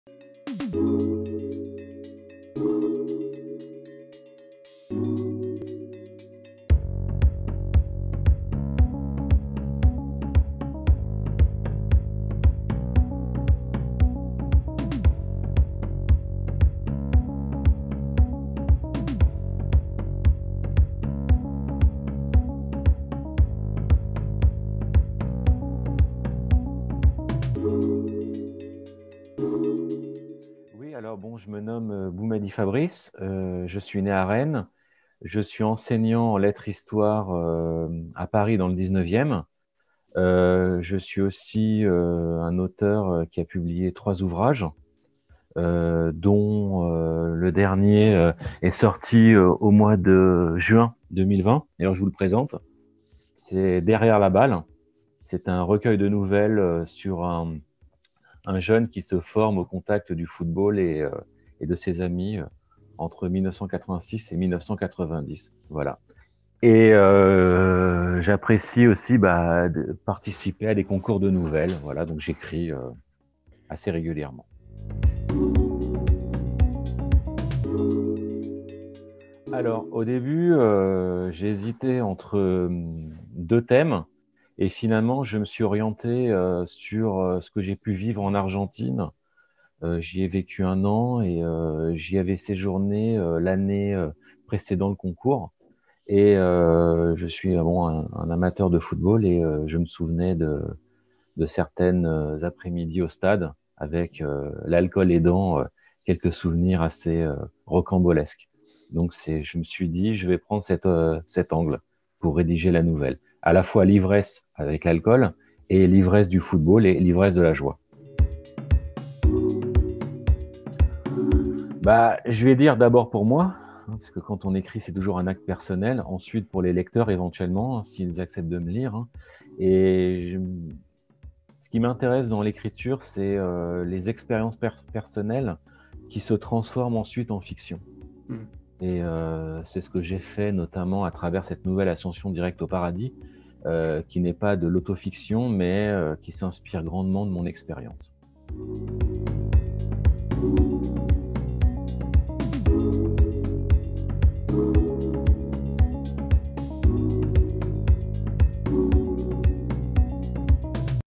INTERVIEW - 3ème prix du concours de nouvelle ACA 2020
Vous aimez écrire ? Le concours de nouvelles de l'ACA est ouvert à tous les membres de la communauté universitaire de Paris 8, aux habitants et travailleurs du département de la Seine-Saint-Denis. À partir d’une thématique donnée (en 2020 « L’ivresse »), un jury (composé d’enseignants, de bibliothécaires, et de libraires) prime des lauréats. Leurs oeuvres sont en plus publiées dans un recueil.

Réalisation vidéo :
Pôle création audiovisuelle